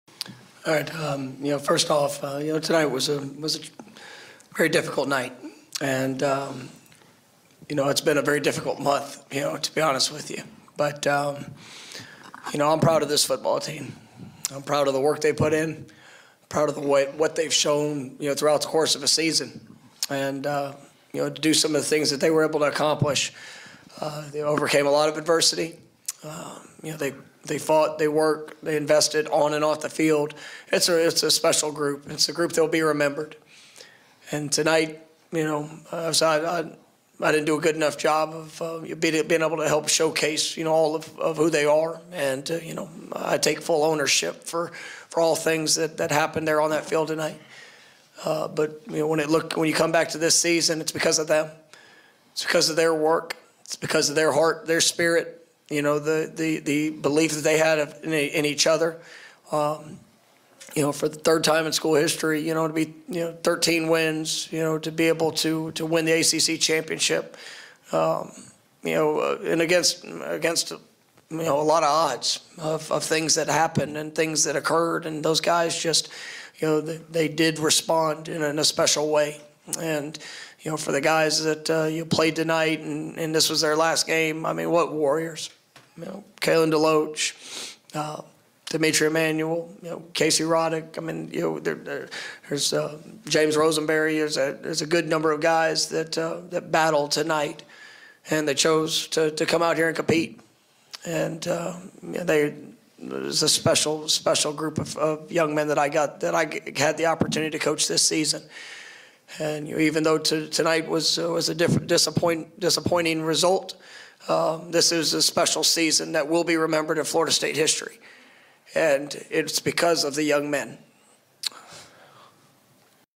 Mike Norvell on Orange Bowl loss: 'I didn't do a good enough job'
Florida State head coach Mike Norvell takes the blame for the Seminoles' showing in the Orange Bowl and praises the team for its championship play all season long.